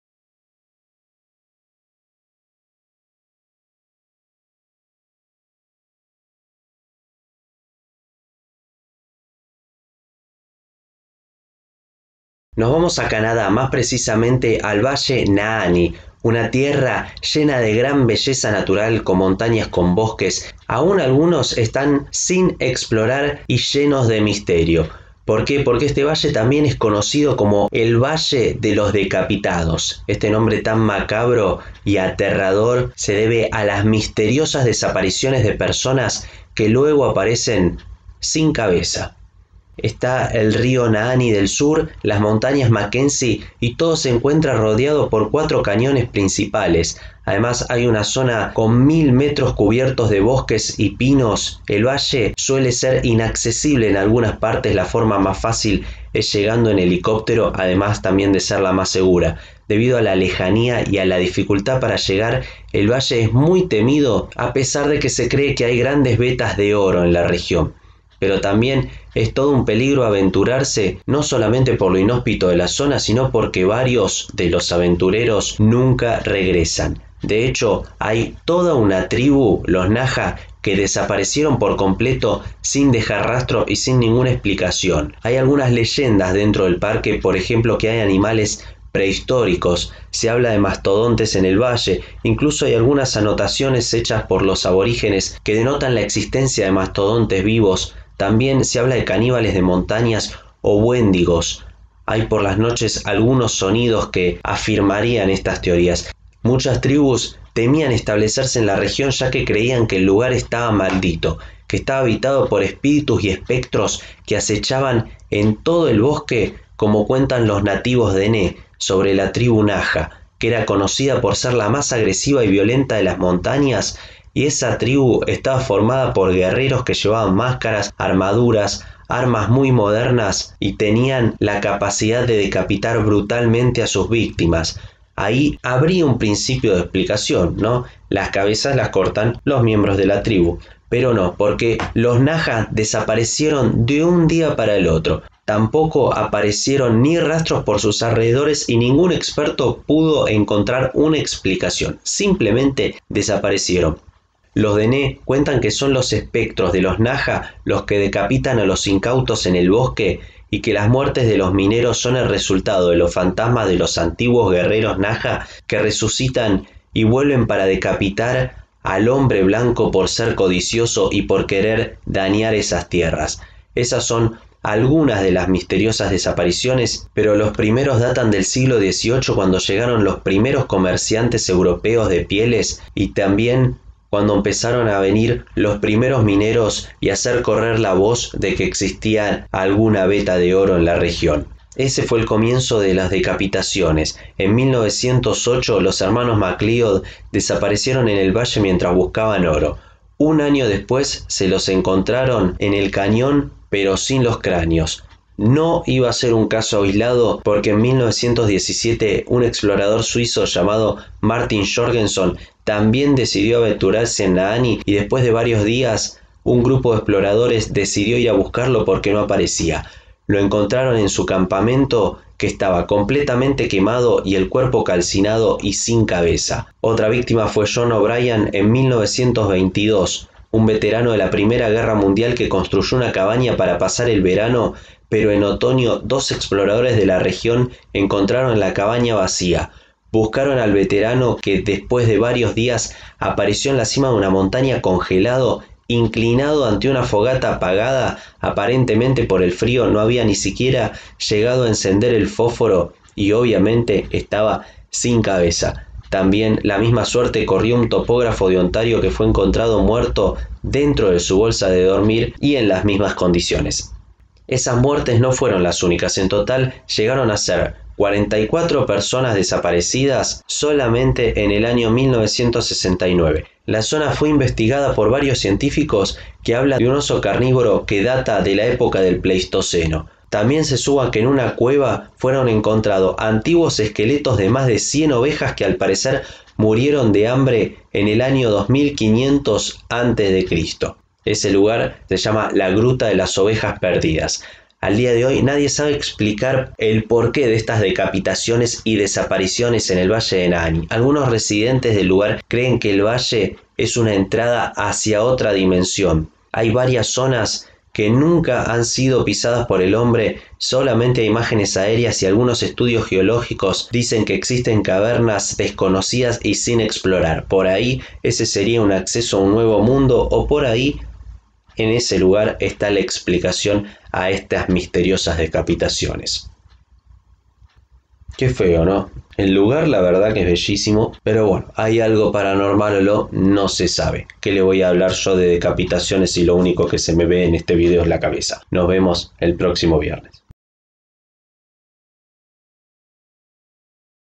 PARANORMAL Ep. 5 NAHANNI, el valle de los descabezados - H. Infaustas Reales #76
Nahanni es un lugar paradisíaco en Canadá. Un valle que sería el sueño de cualquiera. Pero más allá de la belleza del lugar, hay un atractivo muy particular. En ese lugar aparecieron muchos visitantes descabezados. Hay varias teorías, pero la mayoría cree que algo sobrenatural en el valle...

Yenisei96
Video 101